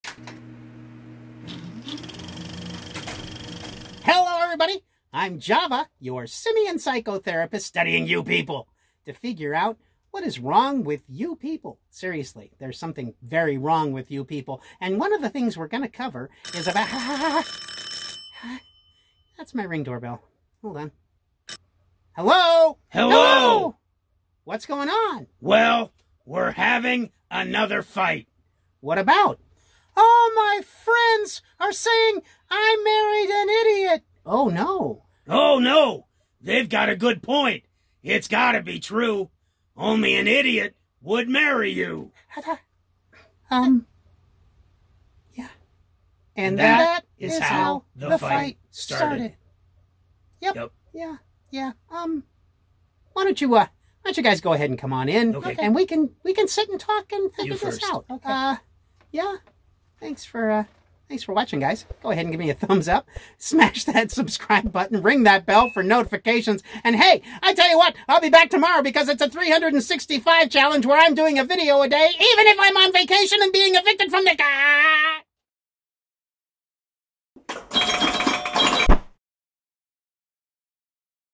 If it's true, it's true
It's Java... he's a chimp, and a psychotherapist.
(what do you get when you combine a pandemic taking away live audiences, an actor with a new puppet @furrifingers, the desire to learn editing and spare time? )
Find outtakes from time to time on Java's instagram  @javathechimp